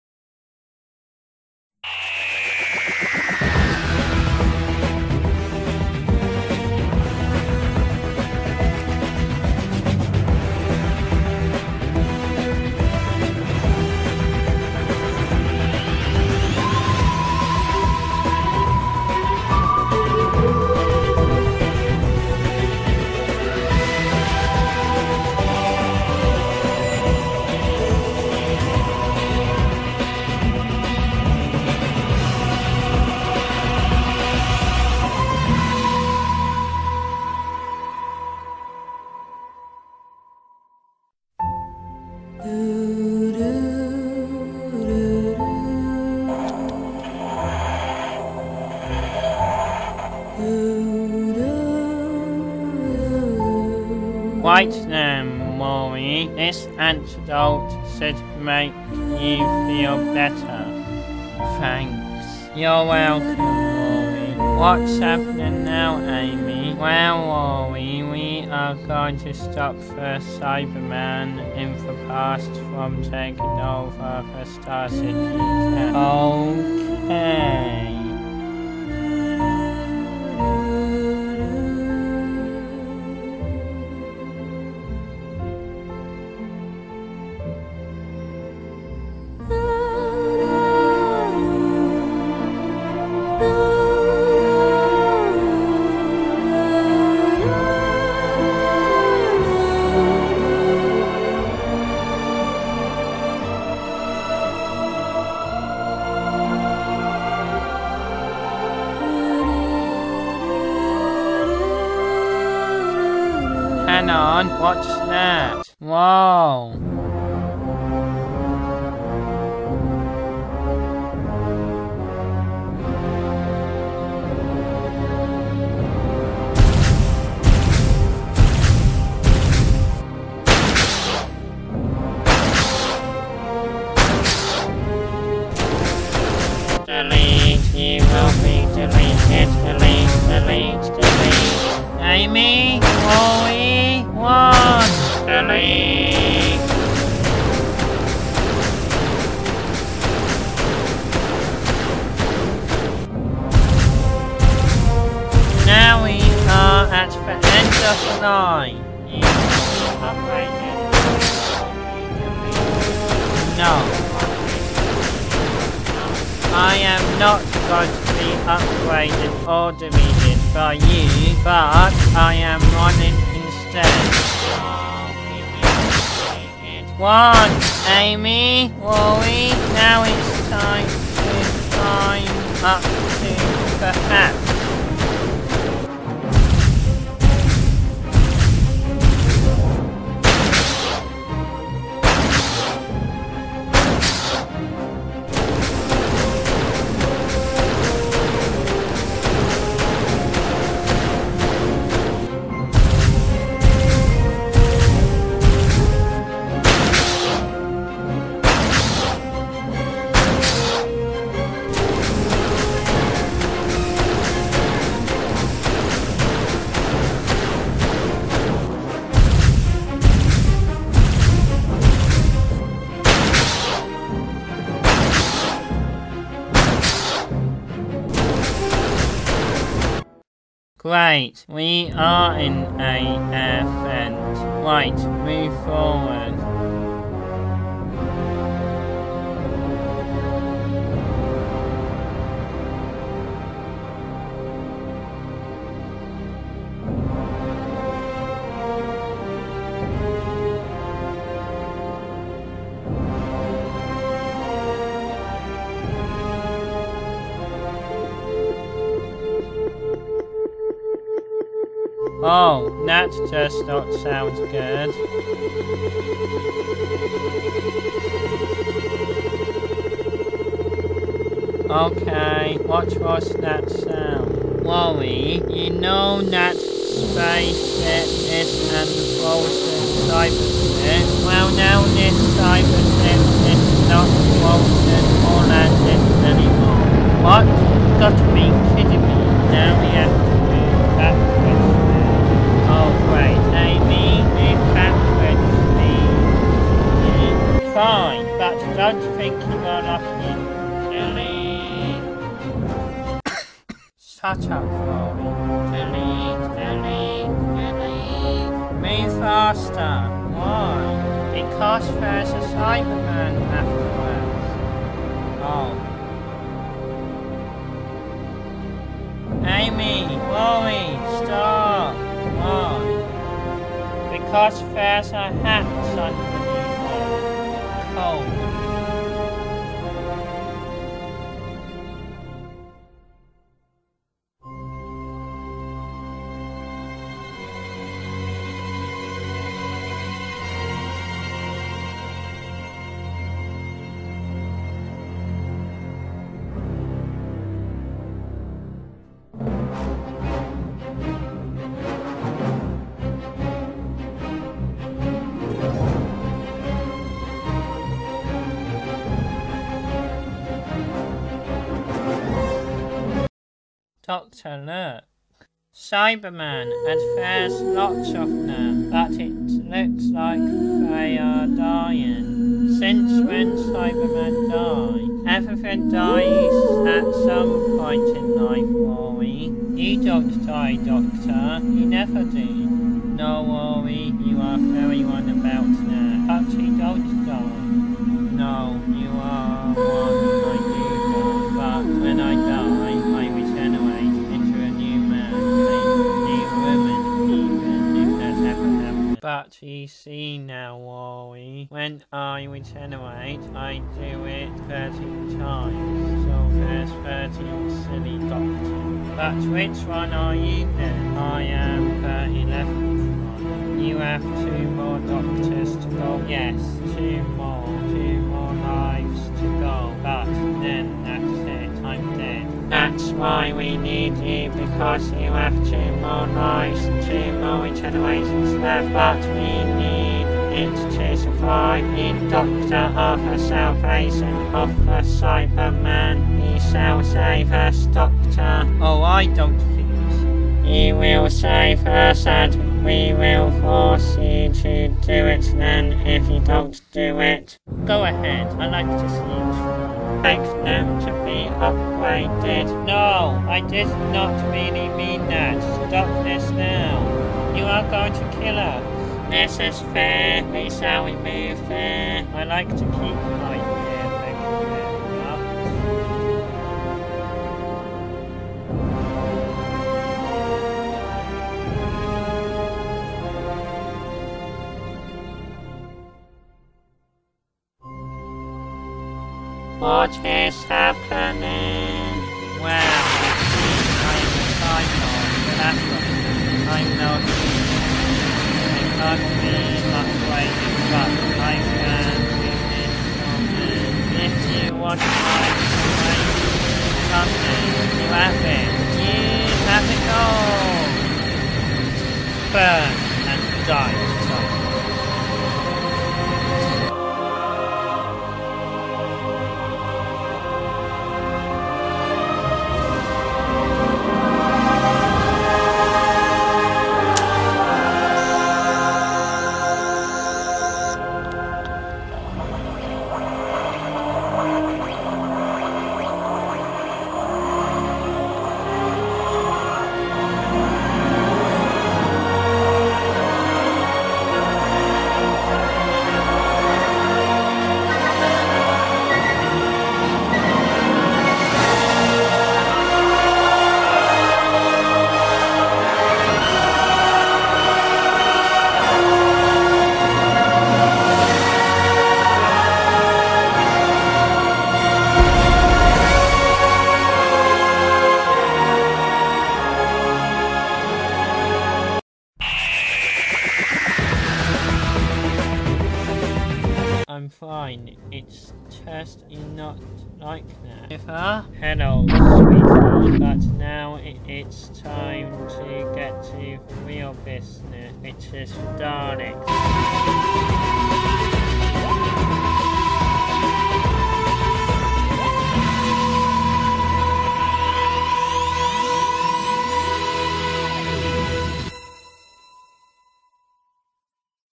|Doctor Who FA|S1|EP 4|The Dying Army|
Now Rory is back and he's recovered,now the Doctor,Amy and Rory have to go back in time to stop,the Cybermen to save the Controller and the Smiler's.

Last Episode/Episode 3